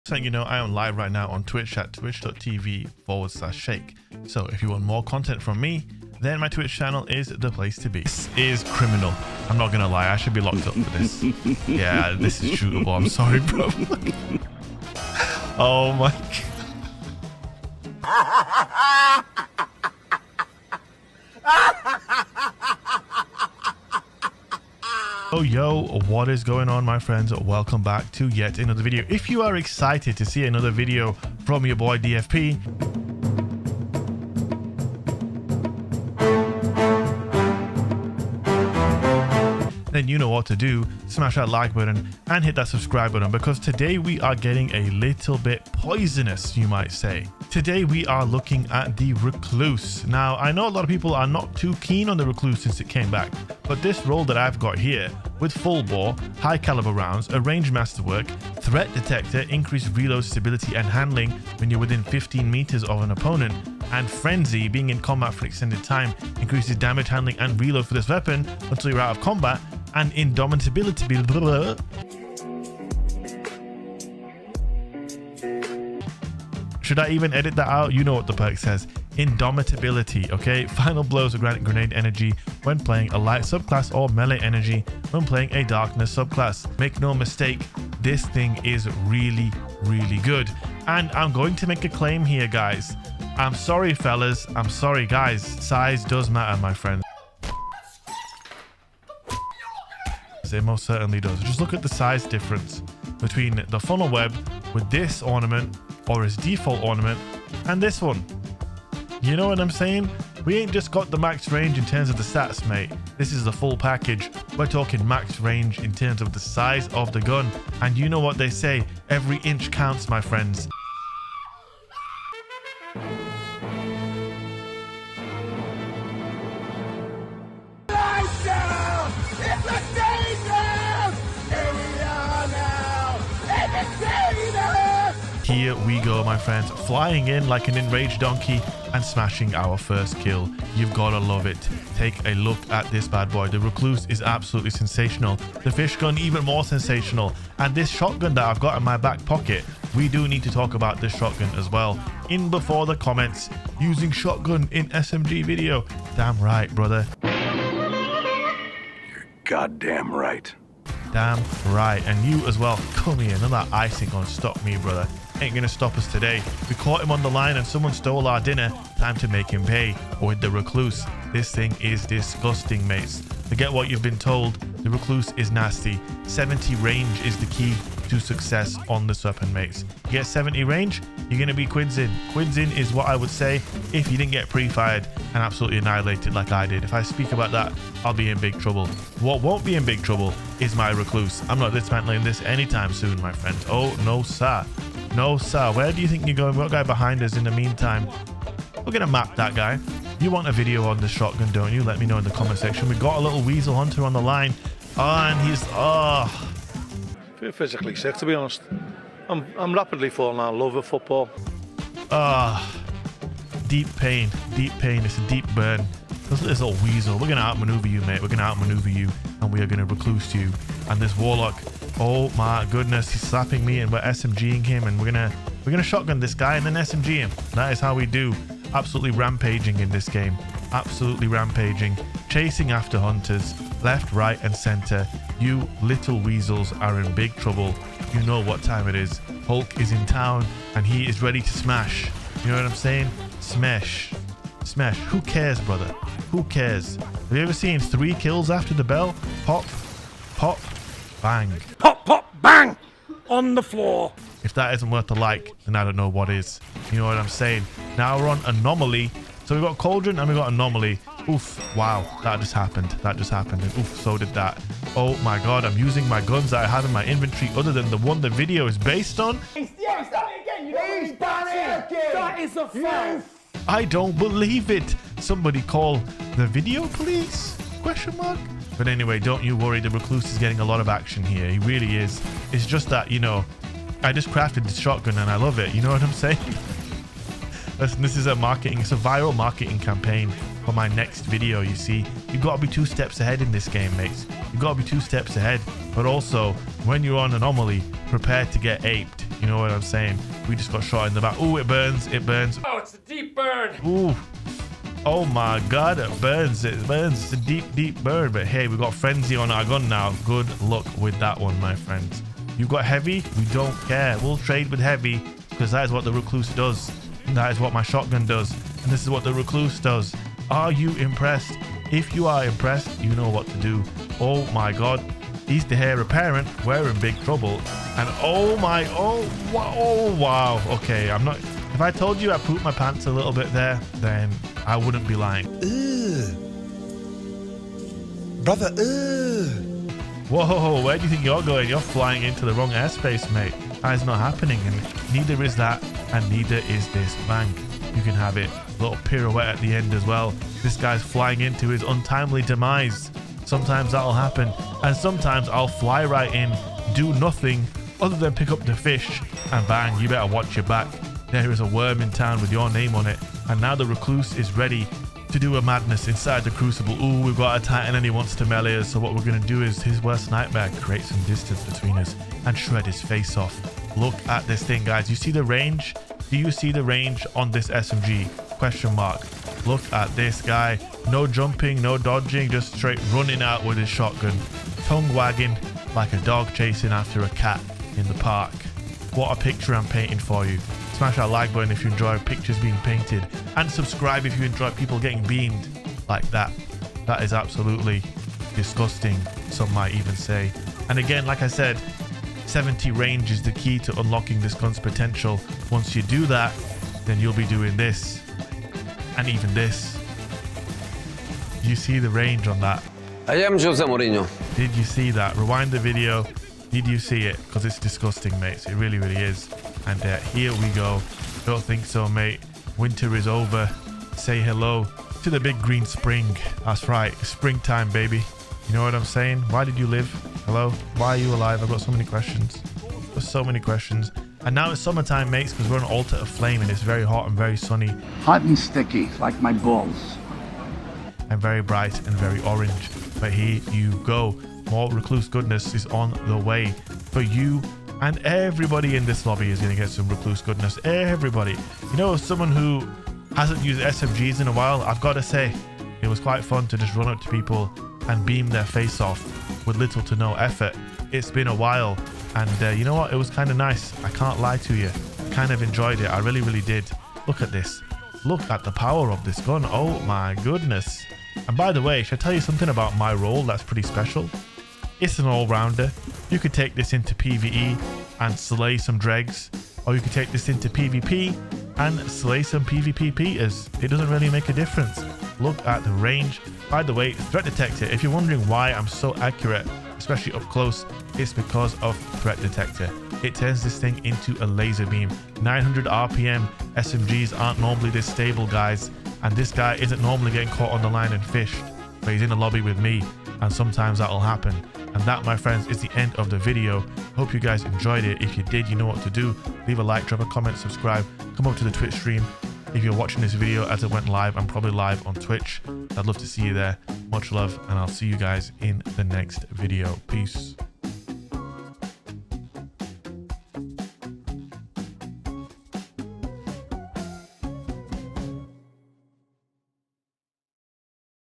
THE MAX RANGE RECLUSE IS PAY TO WIN!? (It Changes The Weapon)
So it turns out, the only way to truly get MAX range on the new Recluse is to use a certain ornament...